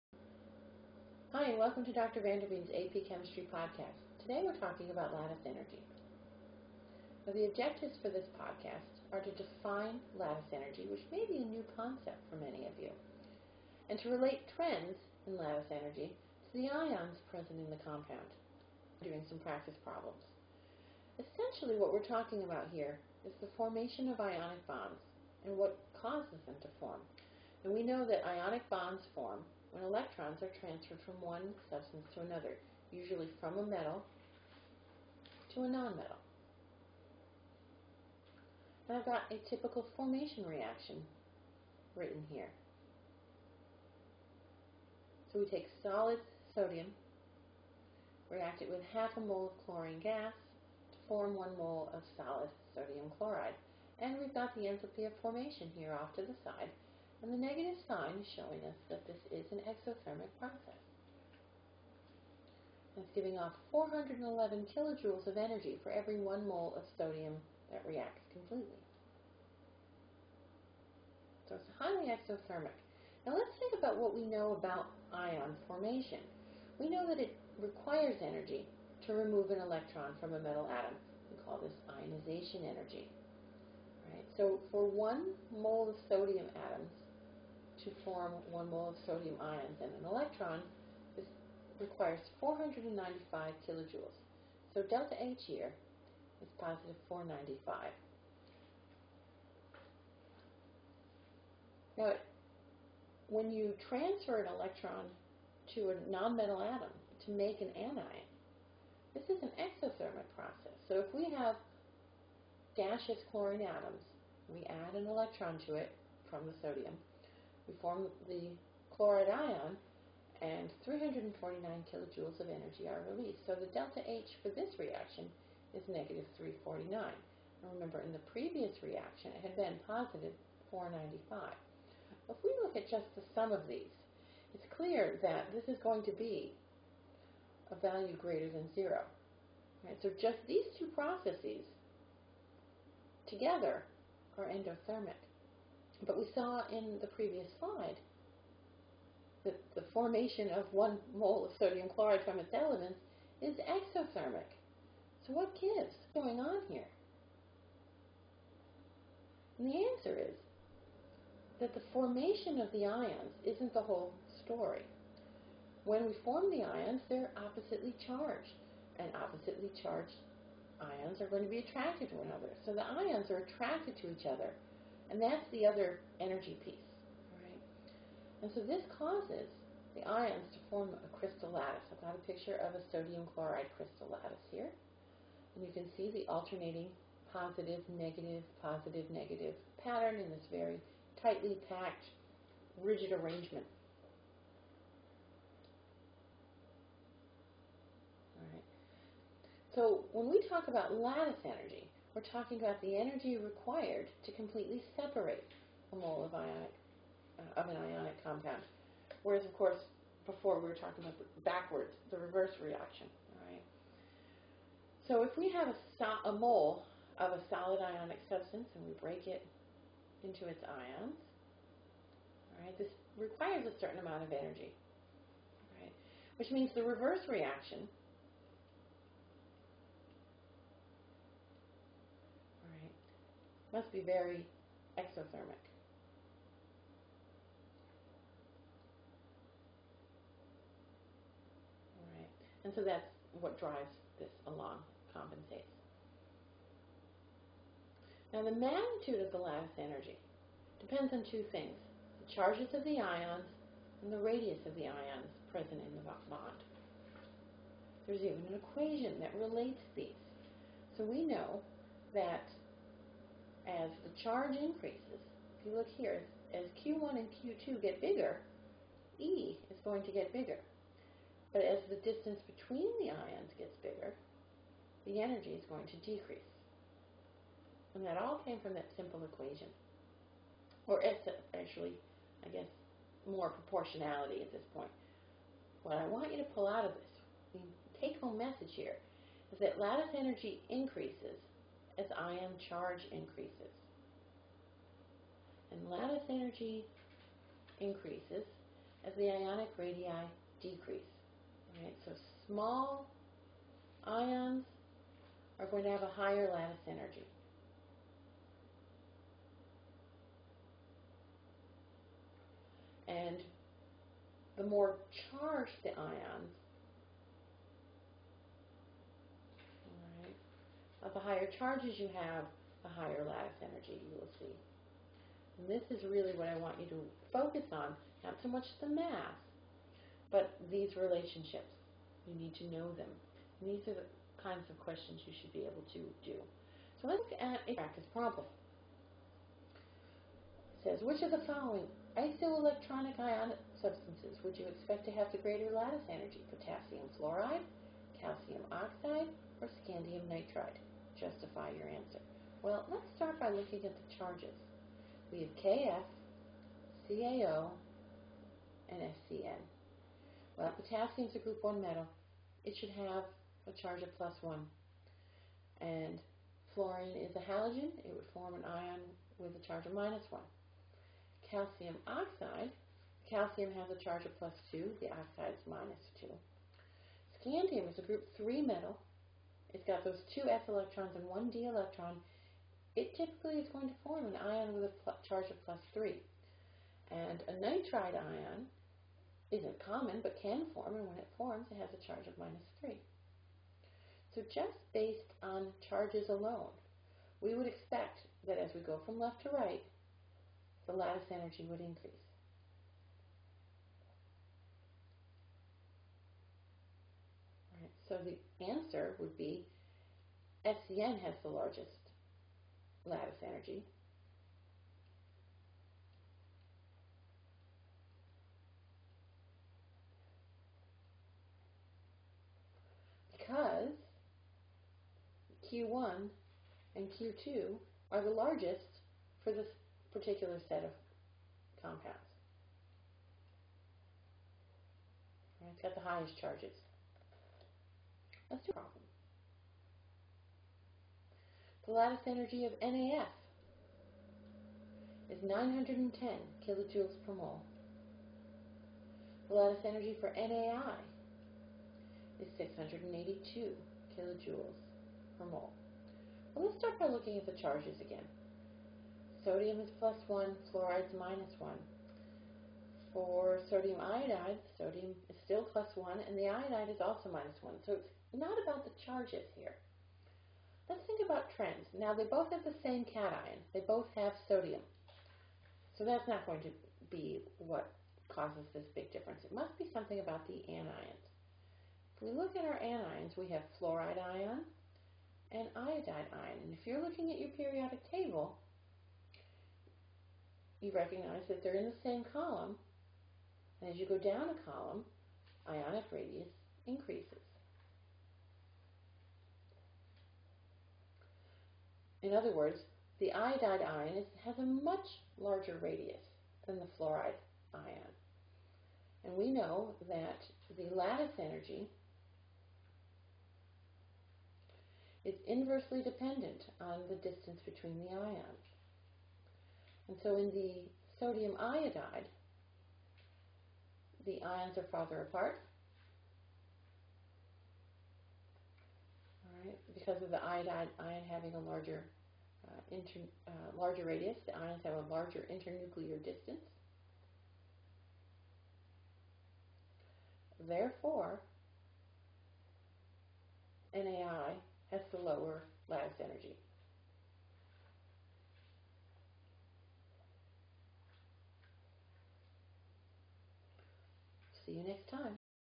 Lattice Energy
Define lattice energy and relate trends in lattice energy to the ions in the crystal.